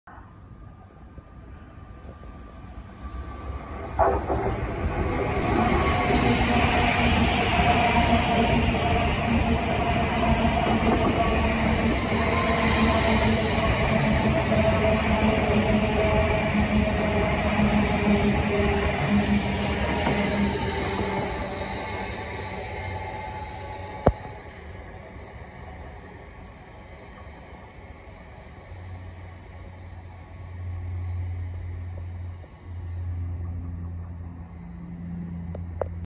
Greater Anglia Stadler Stansted Express Class 745 passing Brimsdown to London Liverpool Street
745109
Greater Anglia
Stansted Express
Stansted Aiprort- London Liverpool Street via Tottenham Hale